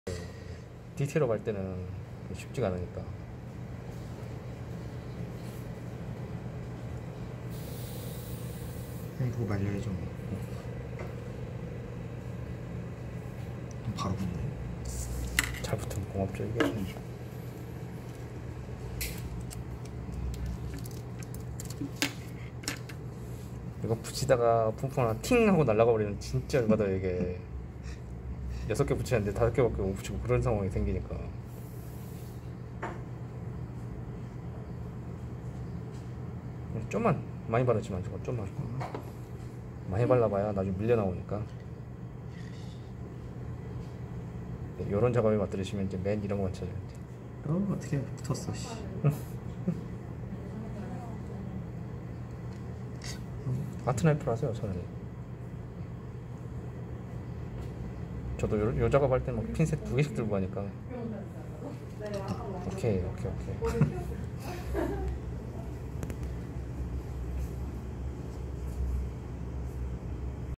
튜닝타임즈 강좌 명동 정화예술대학교 평생교육원 박현준님의 Tamiya 타미야 1/24 Nissan 니산 Skyline 스카이라인 2000 GT-R 엔진 및 섀시 디테일업 작업
명동 정화예술대학교 평생교육원 프라모델 에어브러시 수업 수강생 박현준님이 이번에는 제대로 만들고 싶어 하셔서 설명서에 명시된 색상에 맞춰 부품을 색칠해주셨구요, 한정판으로 키트에 동봉된 금속 부품과 메탈스티커를 활용하면서 디테일업을 해가고 있습니다^^